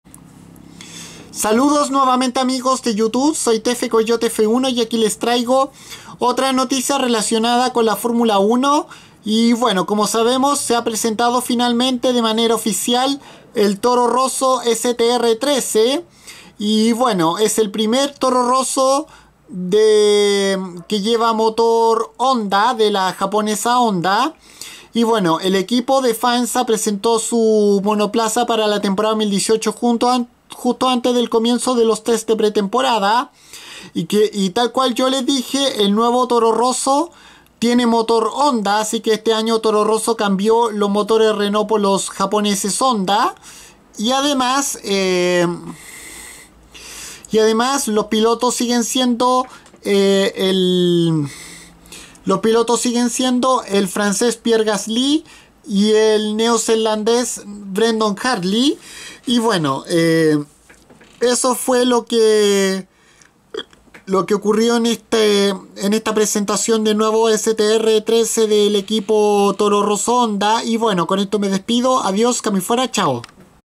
Scuderia Toro Rosso Honda presentó su nuevo STR13 para 2018
El equipo italiano de Faenza ha presentado su arma con la que competirán esta Temporada 2018 de la F1 y que es el primer auto de esta escuadra en tener motor de la nipona Honda